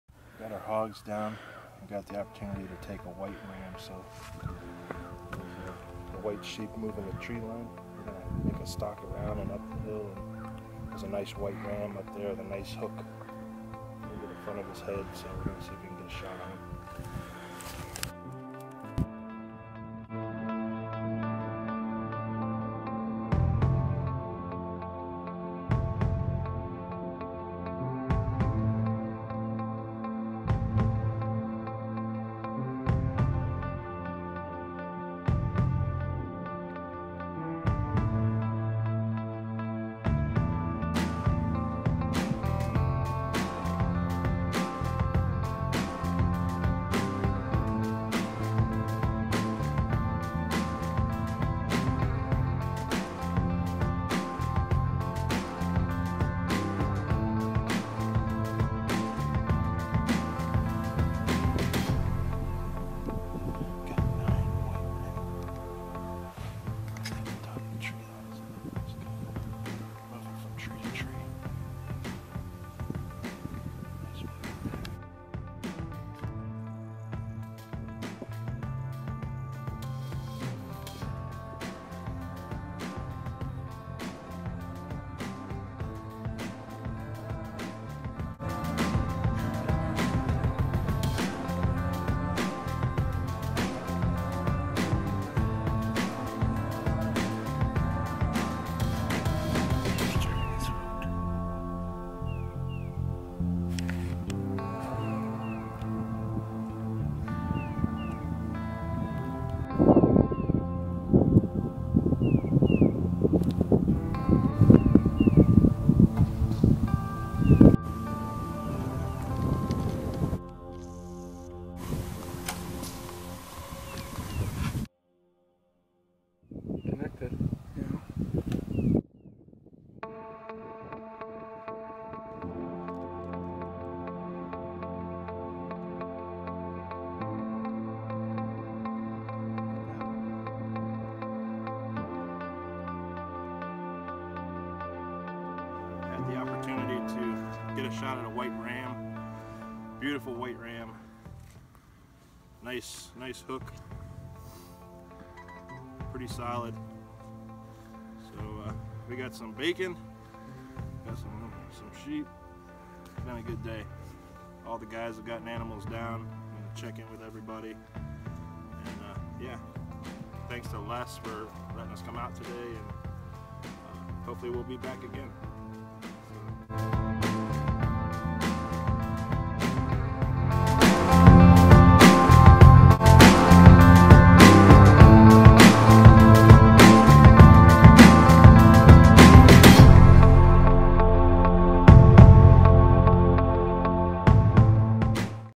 2017 6 yo "RAM" WY. Bowkill
Mike Takovich, Antler Assassins TV Host/Producer Started This Stalk At Around 250 yds. From Semi-Flat Ground Of Wyoming To The Heard Of Sheep. Using Trees As Cover Until He Could Reach The Downslope And Make A Slow Stalk To Finish His Shot At A Close 41 yds. "when I popped over the hill they weren't quite sure where to go or where He came from, but made a nice clean and quick ethical killshot. and recovered the animal about 70 yds from impact" -MIKE TAKOVICH